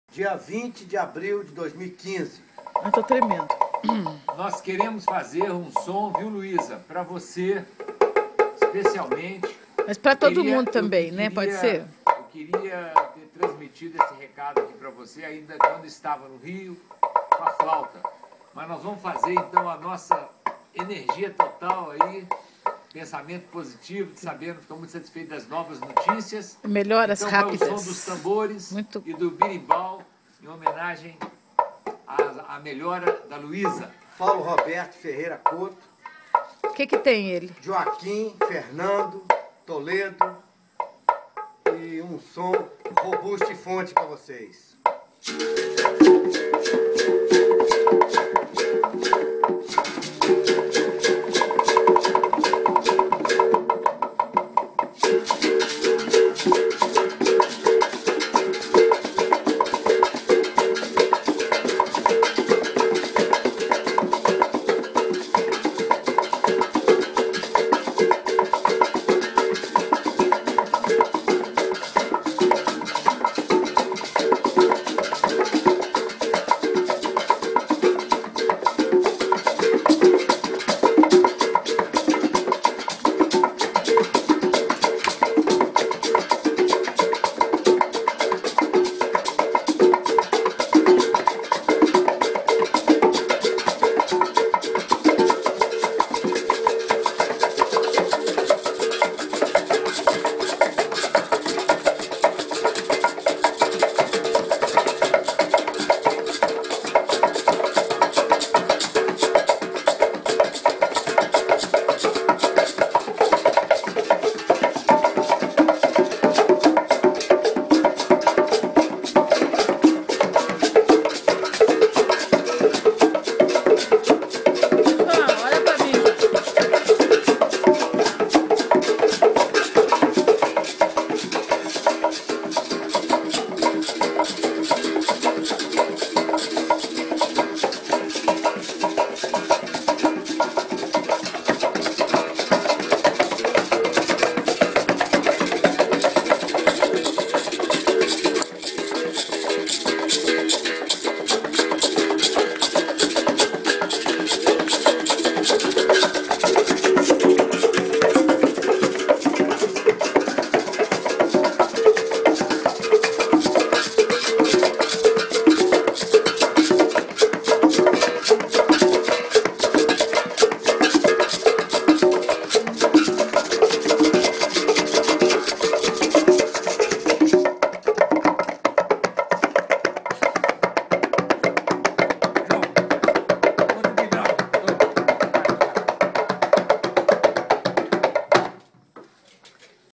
Berimbau, Bongô, e Congas: Mestre Polêmico, Henrique, e Estêvão. Tiguera 363. 20h34. 20abr15
Berimbau, Bongô, e Congas, com João, Henrique e Estêvão Couto Teixeira no Condomínio Tiguera. A corda Arrebentou. Homenagem a Luiza Robusti Fontes, Paulo Roberto Ferreira Couto, e Joaquim Fernando Elizeu Toledo. Registro de Maria do Carmo. Juiz de Fora, Minas Gerais - MG, Brasil. IMG 1523. 20h34. 20 de abril de 2015.